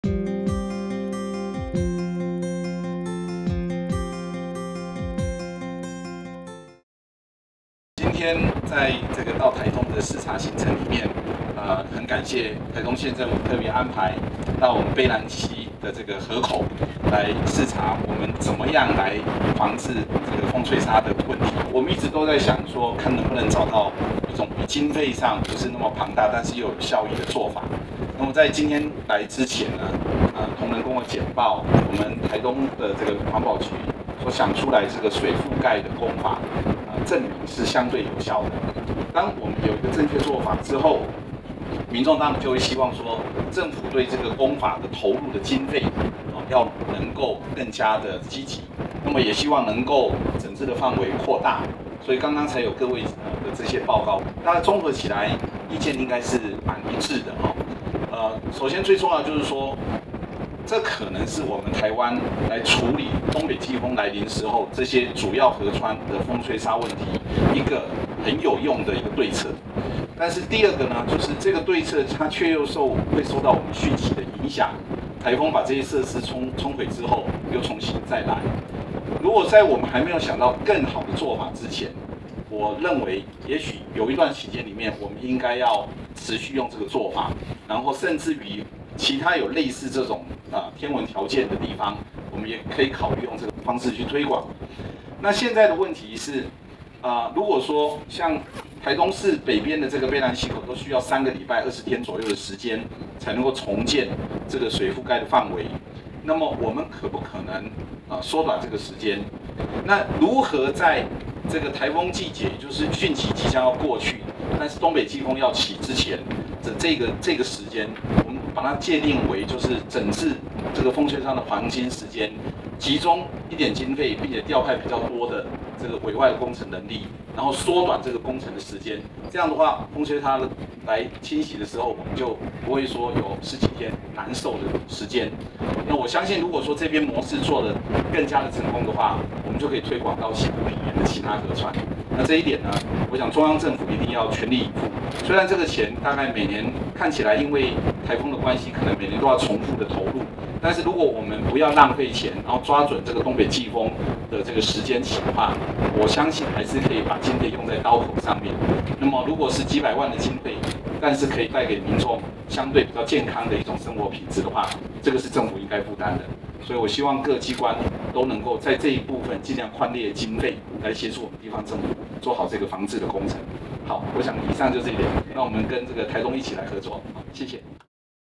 行政院長江宜樺視察卑南溪風飛沙改善情形
行政院長江宜樺今（2）日親赴臺東縣視察「飛行學校」、「卑南溪河川揚塵(風飛沙) 防制情形」等2項地區建設計畫，針對「熱氣球經常起降區域電桿地下化」構想方案，除指示交通部成立專案小組積極規劃，並逐年編列經費，以推展觀光產業之外；同時責成經濟部儘快進行跨部會合作，提出改善卑南溪揚塵更精進有效的水覆蓋工法，以提升民眾的生活品質。
在聽取「卑南溪河川揚塵(風飛沙) 防制情形」簡報後，對卑南溪揚塵防制工作成效表示肯定與支持。江院長強調，水覆蓋工法對於處理東北季風來臨時主要河川風飛沙是一項有效對策，希望在颱風季節汛期過後，東北季風開始前這段整治風飛沙黃金時間，透過集中經費、調派委外工程人力、縮短工程時間，以減少風飛沙侵襲。

江院長除指示相關單位寬列經費，並責成經濟部為召集機關，與環保署、農委會等部會與縣府進行跨域治理，將已經證明相對有效的水覆蓋工法，加以精進改良，並使它更具效力，讓民眾受到懸浮微粒風沙困擾的時間縮短，待此模式更為成功有效後，再推廣至西部平原其他河川防制工程，期望藉此帶給民眾更好的空氣品質與生活環境。